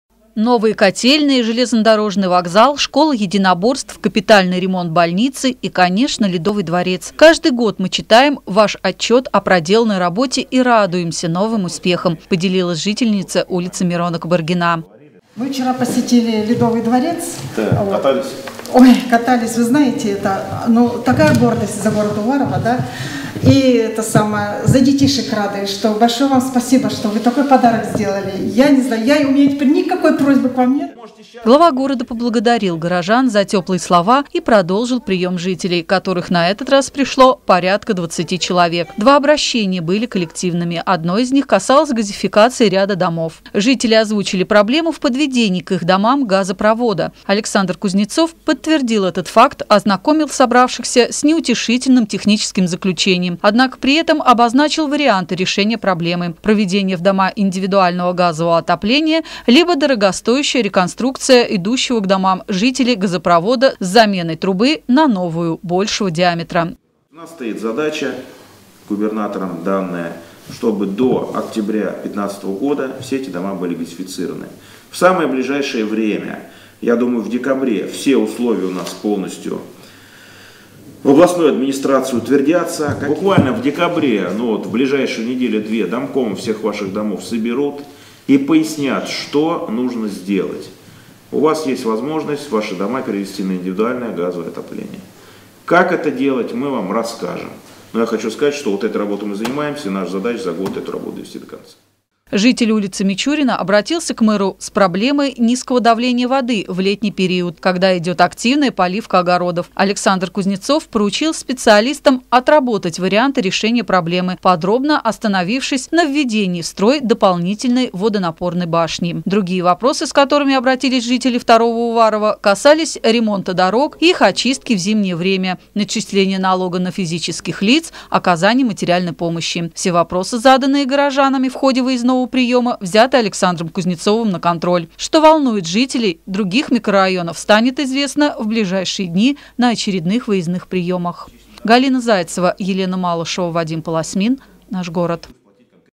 5 12 14 vyezdnoj priyom vo 2 uvarovo
С благодарственного письма в адрес главы города начался прием жителей 2-го Уварово 4 декабря. Семьи Кравцовых и Востриковых вместо привычных просьб поздравили Александра Кузнецова с наступающими новогодними праздниками и озвучили слова благодарности за возведение на территории города социально-значимых объектов.